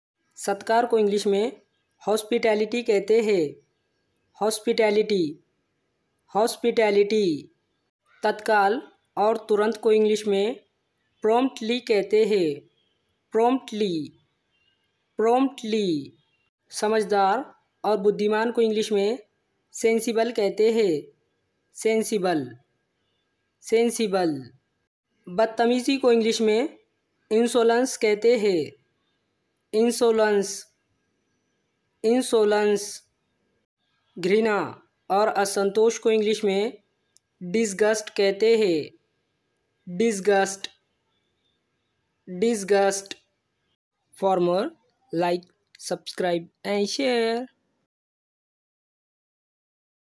New english vocabulary | नई अंग्रेजी शब्दावली
New english vocabulary | नई अंग्रेजी शब्दावली |‎@Spoken English

25 daily use english sentences with Hindi and Marathi meaning I रोज़ बोले जाने वाले इंग्लिश वाक्य हिंदी और मराठी अर्थ के साथ | part 3